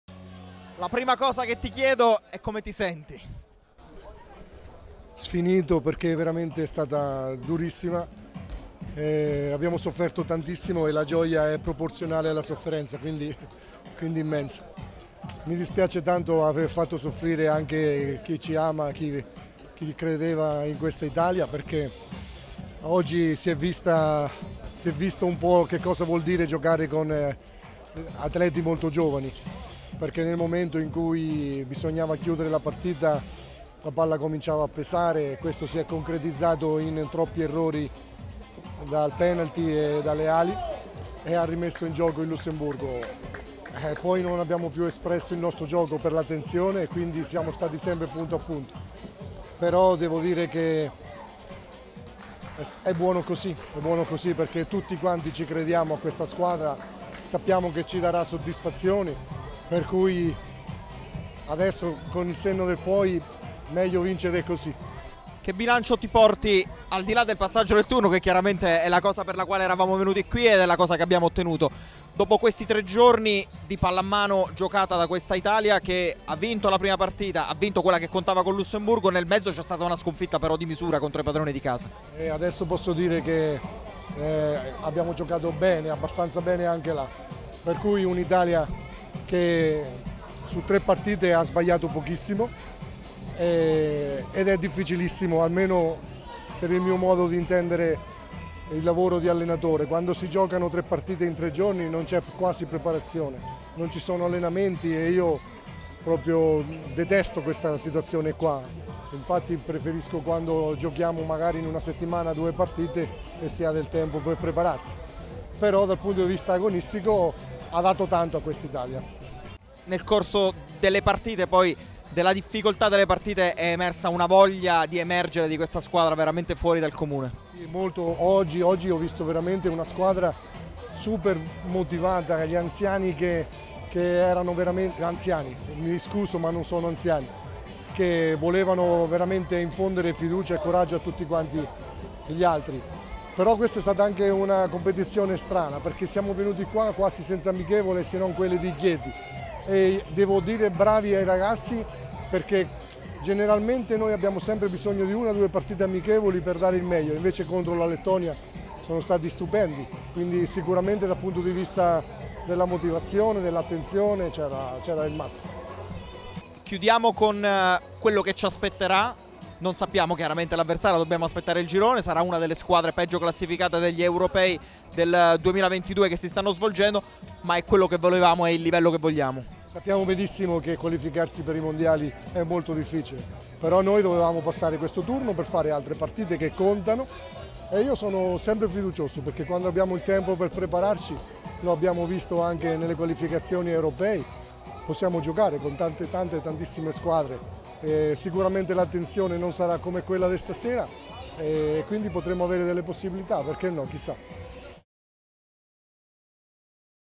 QUI ITALIA | Azzurri ai play-off Mondiali: il post-gara con Riccardo Trillini
🔵 Qui Torshavn 🎙 A fine gara, dopo avere centrato l'accesso ai play-off di qualificazione ai Mondiali 2023, è grande la gioia per il direttore tecnico azzurro Riccardo Trillini.